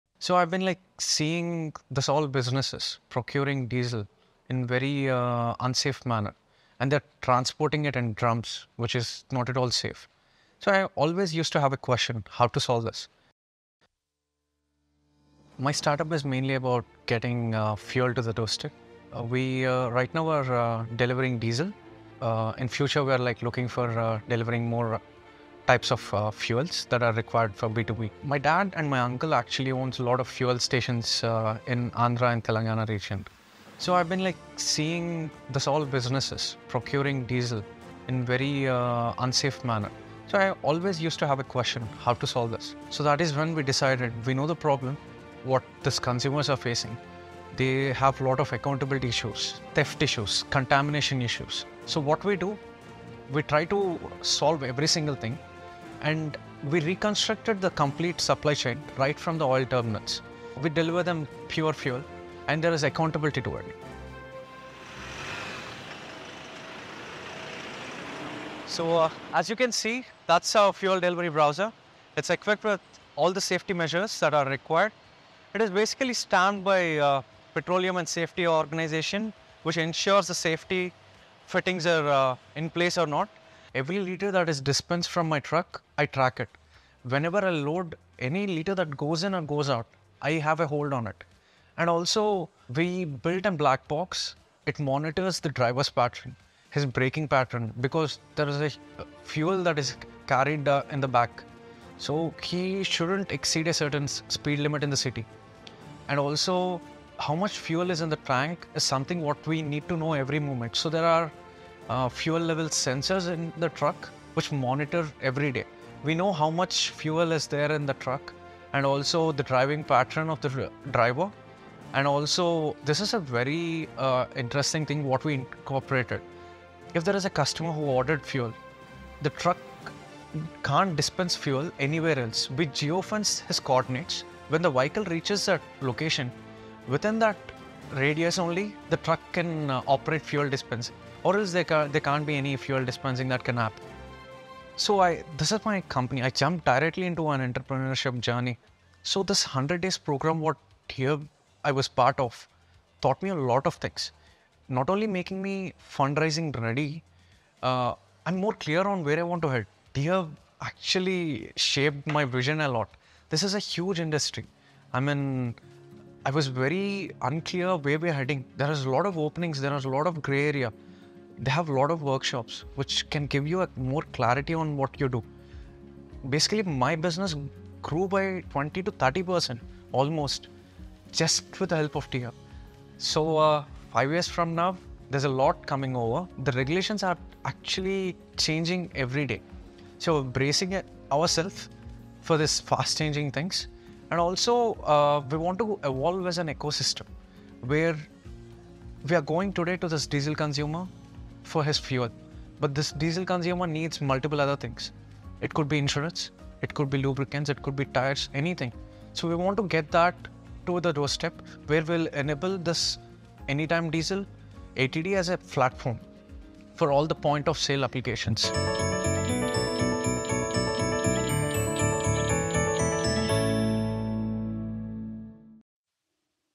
India's Tech-Driven D2C Diesel Delivery Platform | Anytime Diesel | T-Hub Founder Talks
Procurement, transportation, and monitoring of diesel is a challenge. A lack of tech and accountability causes fuel theft and even contamination issues.

A T-Angel graduated startup is innovating & restructuring the fuel delivery industry with its tech-driven approach.

Join its Founder & CEO, Rahul Reddy K., as he talks about his revolutionising platform with fuel tracking, driver monitoring, and geo-based fuel dispensing.